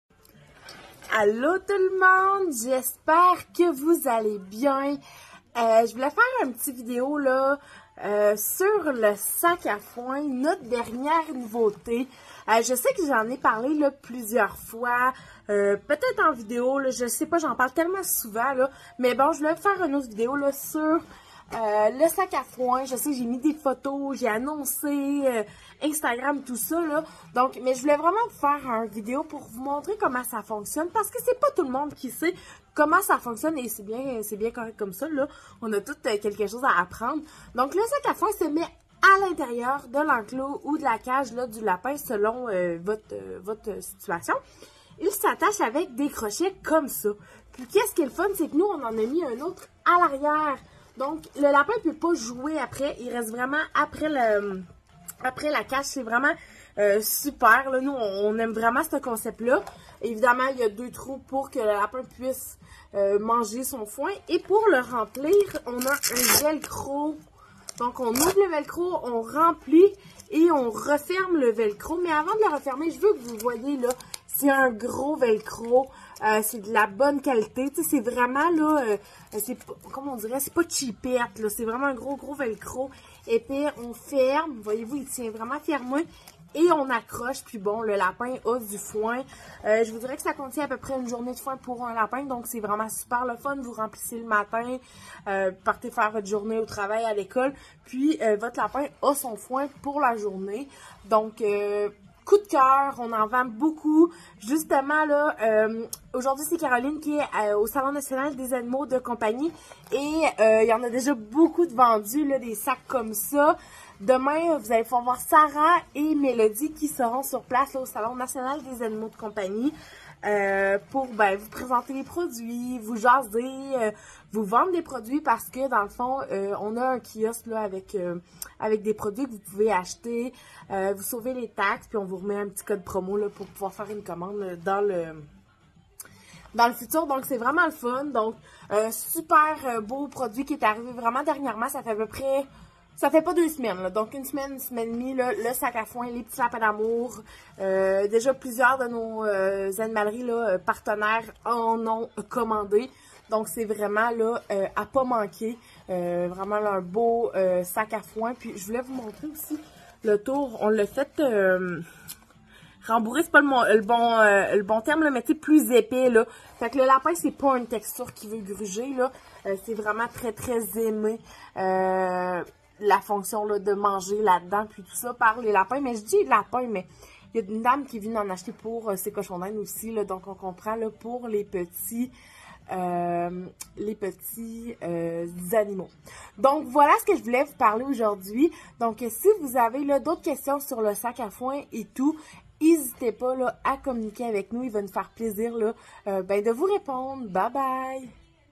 Comment faire manger plus de foin à mon lapin?
Cette question revient très souvent. :)
Le sac à foin permet de réduire le gaspillage et permet aux lapins de manger plus de foin.